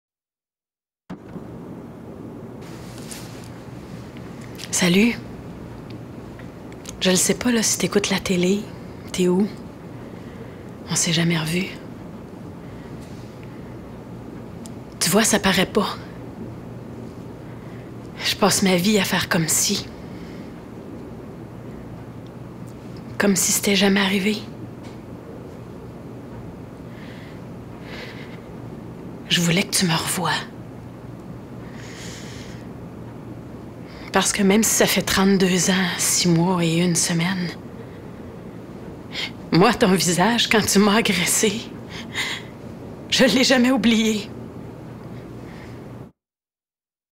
Dominique Lamy - Publicité contre les agressions sexuelles ( MCCCFQ)
Publicité choc
Comédienne Dominique Lamy